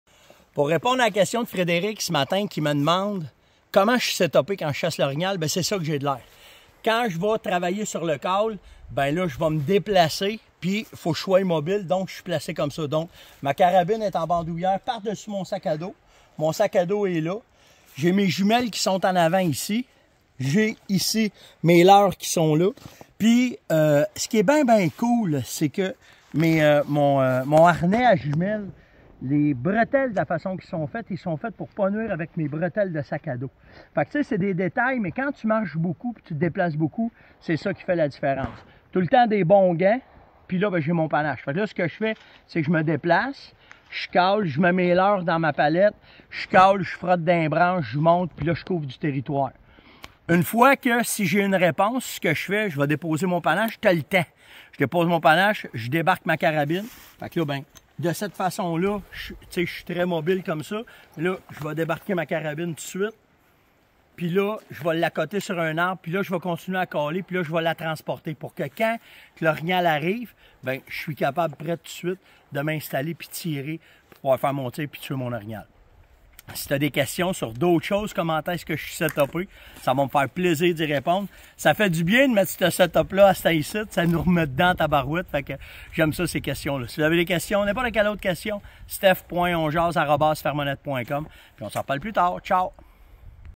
Comment je suis équipé pour caller l'orignal
Comment tu est organisé avec ton sac à dos, tes jumelles, ta carabine et ton panache quand tu chasse l'orignal?